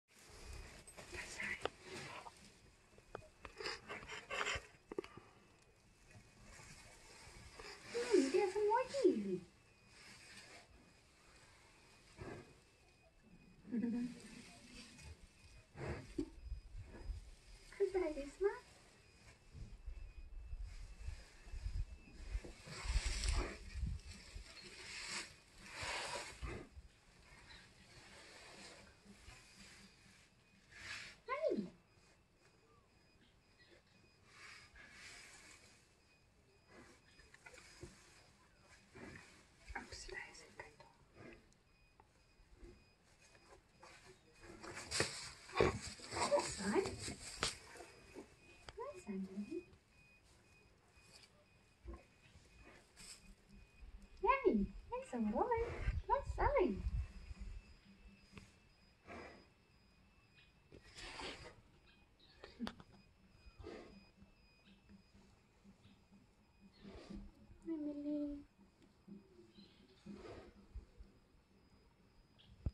When Esme met Mielie
When Esmé met Mielie … A FlashbackFriday to 2017 when a little lamb called Mielie first arrived at HESC to keep Esmé company. Watch how Linri introduces them to each other. Mielie was skittish at first, and Esmé being Esmé, made sure she didn’t lose out on any attention from Linri. Now look at these two and the friendship that has developed!